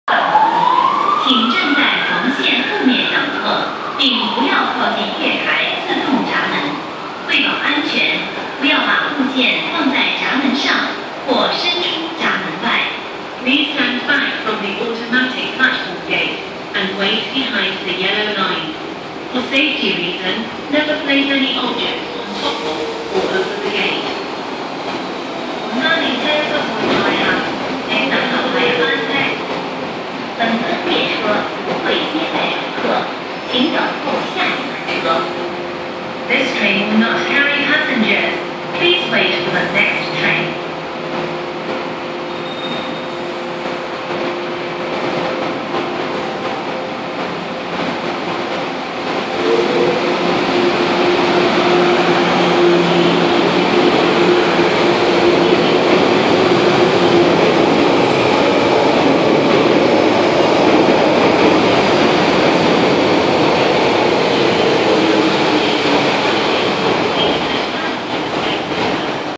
【終於捉到097喇】港鐵 KTL M-Train (A258/A273) 不載客駛經牛頭角站一台
拍攝日期：28/3/2013 (四), 車序：097 (試車)

呢抽落馬洲車傳說今朝早繁跪低左, 佢本身行個序又真係轉左車, 即係大機會瓜左啦。今日晚間時候過九廠發現佢碌緊上出廠線, 應該係整好出黎試車喇, 於是就梗係要捕到佢啦。

本片版權屬Edward Hong擁有, 如需使用作公開或私人用途, 請先得Edward Hong同意和批准, 違者必究.。
You are not allowed to make use of this video without Edward Hong's agreement and permission.